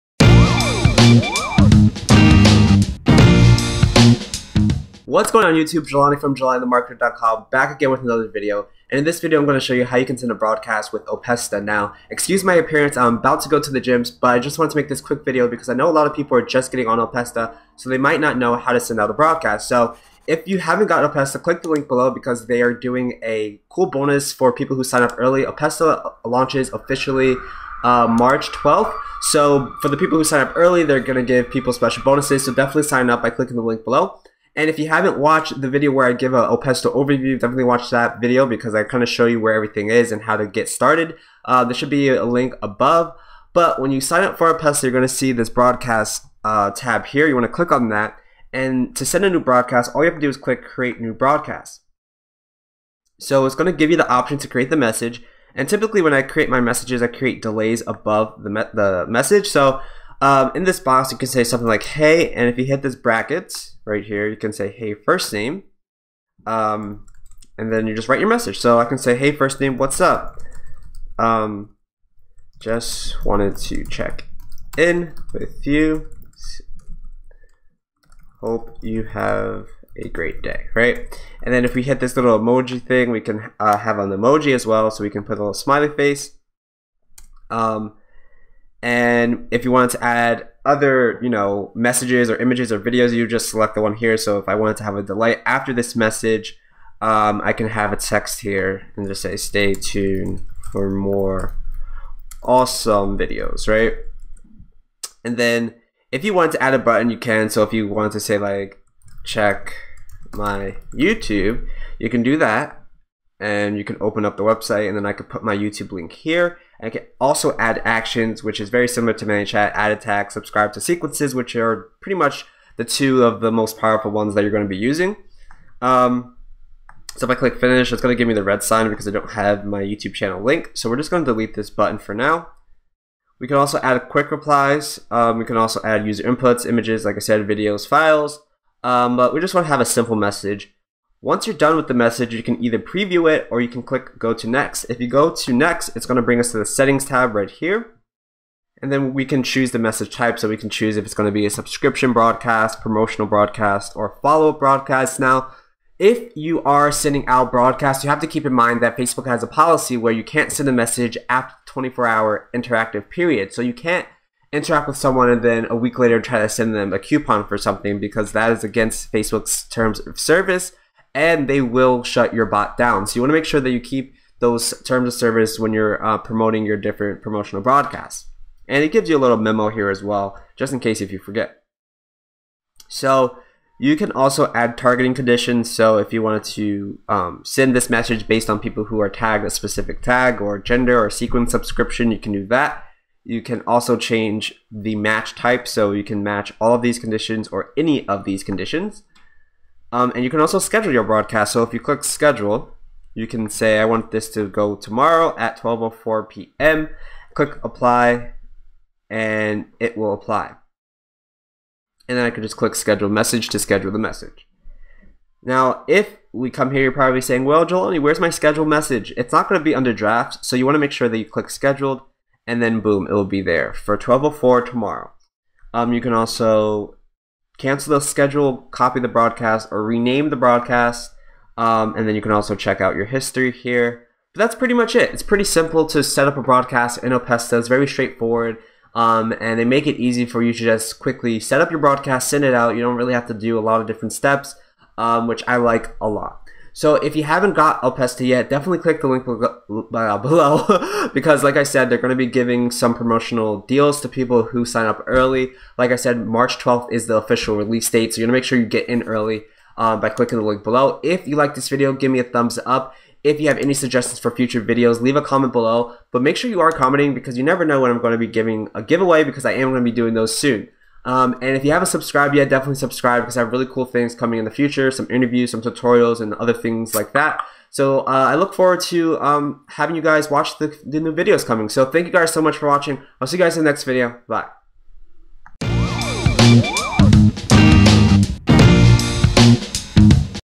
How To Send A Broadcast Using Opesta
In this video I show you how to send a broadcast message using Opesta.

Opesta is a facebook automation tool that makes it easy to send out messages based on the actions people take.

Sending broadcast is just the start of how powerful Opesta can be...and is easy to get those messages set up.

To begin I show you how to access Opesta broadcast tab in the Opesta dashboard.

Once you are in the Opesta broadcast tab, you want to select Create new broadcast and from there I show you how you can send your first facebook messenger bot message.

Opesta makes it easy for anyone to start sending broadcasts to their audience, it's very straightforward which I like alot about Opesta.

Jelani from Jelanithemarketer.com

💥 How To Make YouTube Thumbnails (With Mediocre Photoshop Skills): Video Link

I do make a commission, however, no extra cost to you.